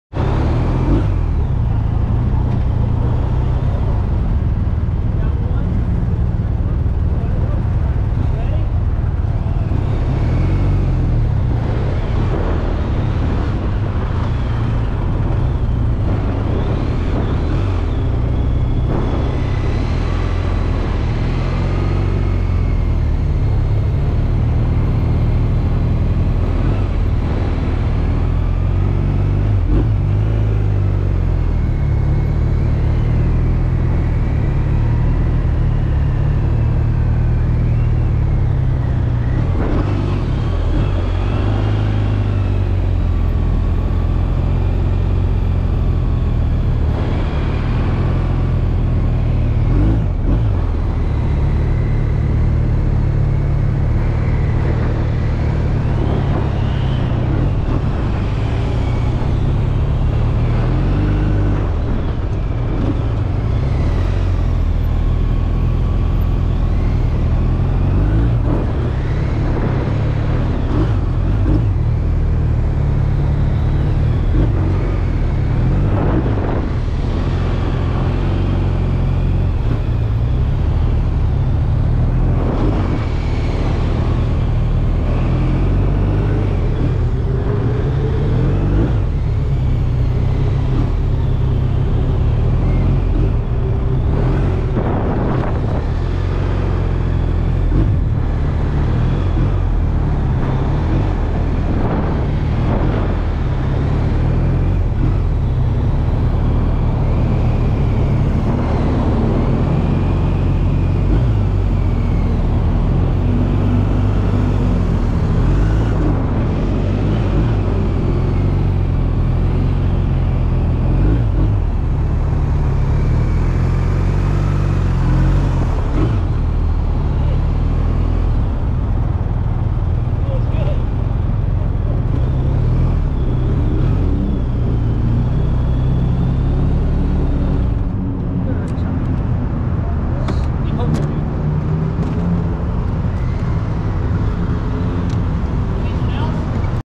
Following / chasing a motorcycle officer with my 2019 bmw r1250rt Ride to live Hawthorne PD
Ride to Live is a Hawthorne Police Department sponsored training class for any motorcycle rider seeking to improve his/her riding skills, enhance their safety and control, and learn the techniques taught at law enforcement motor schools.

This class works because the skills taught are real-world skills.  No classroom! It’s all about “hands on” learning techniques and skill sets that you can put into practical use immediately.

The course is taught by certified police motorcycle instructors and motor officers from the Hawthorne Police Department in an atmosphere that facilitates open communication, lots of interaction and an easy going environment where learning valuable skills in a fun way is key. Police motor officers and instructors from the Torrance Police Department and the Redondo Beach Police Department also instruct as well and they have been a great help in instructing as many riders as we can!